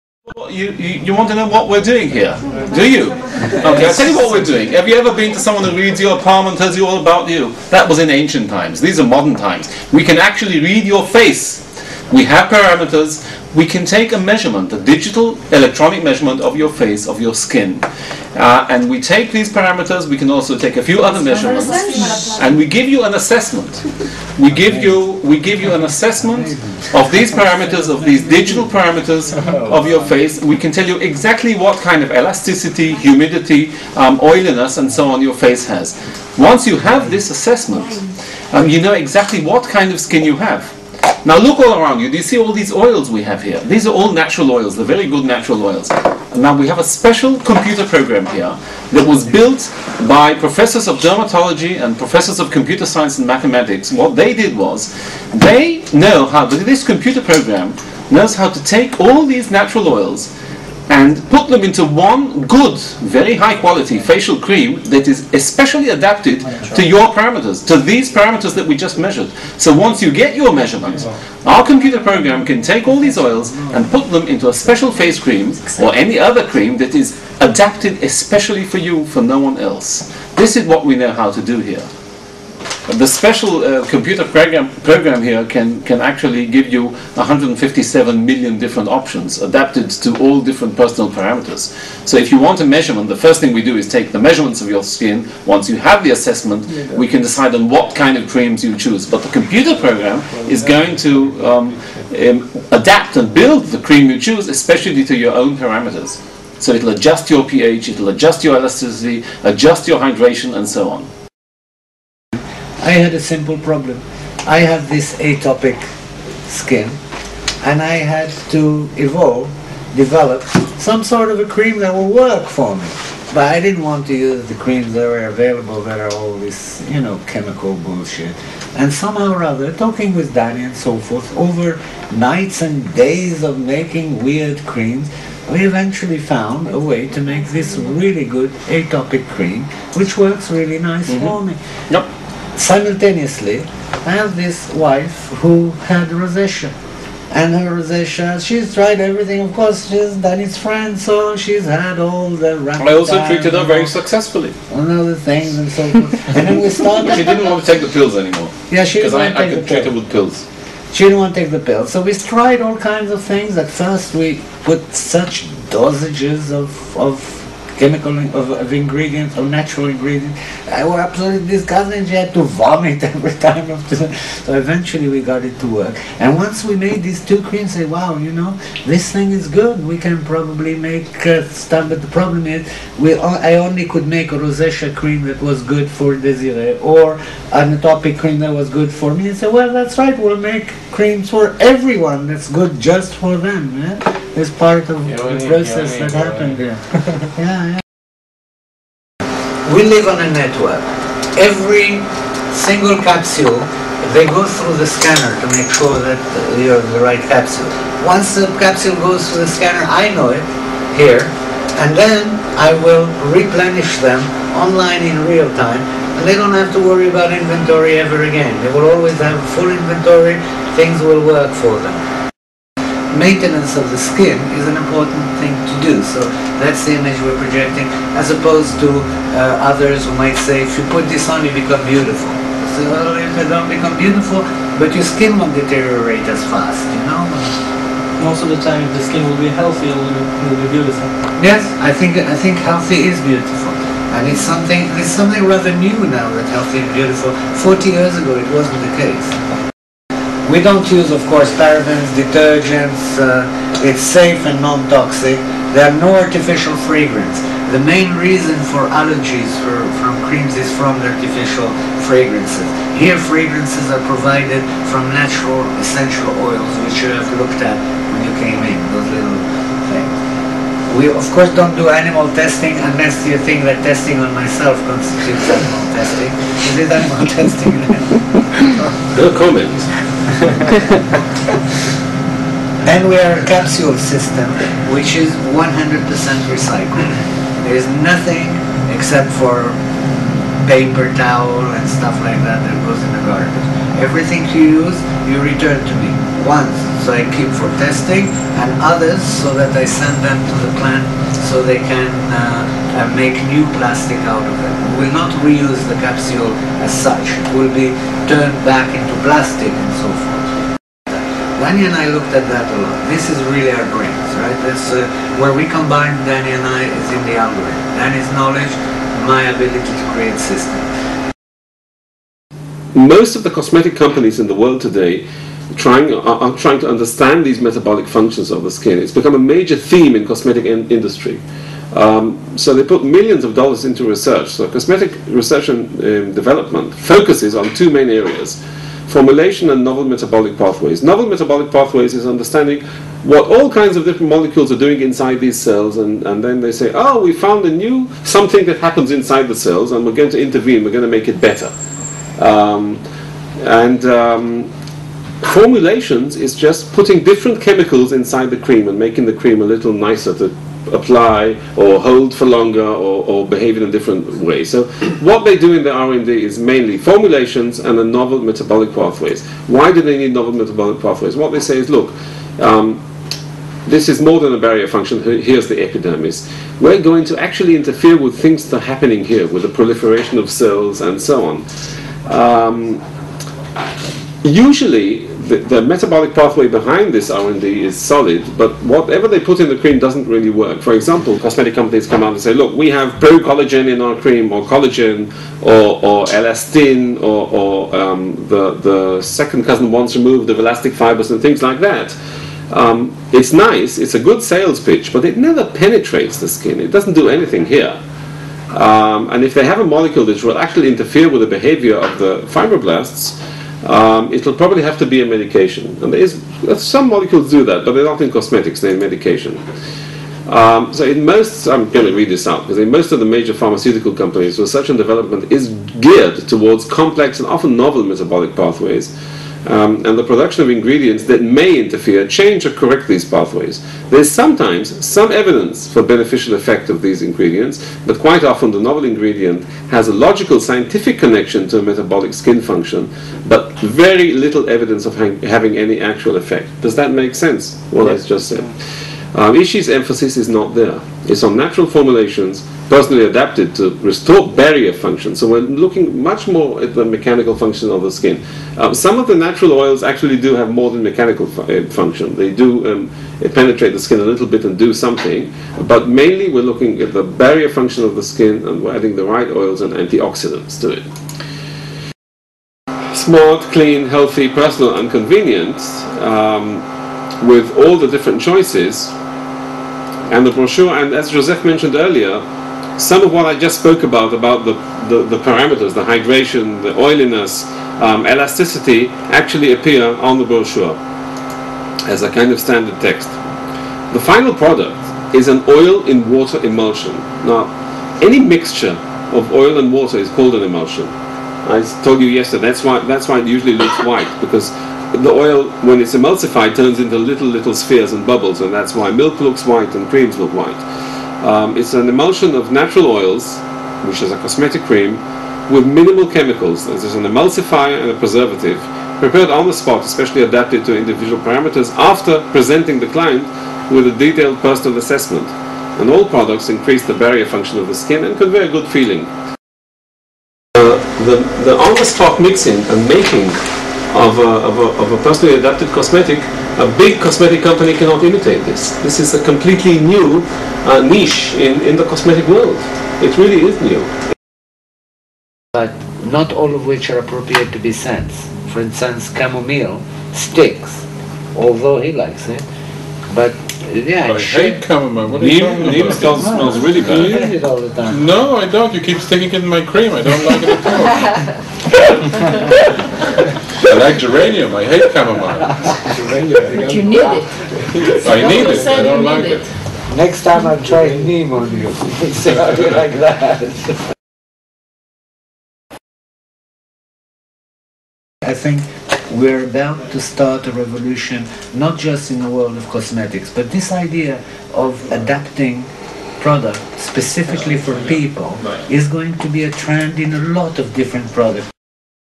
אישי קוסמטיקה
דני וג'וזף מדברים על אישי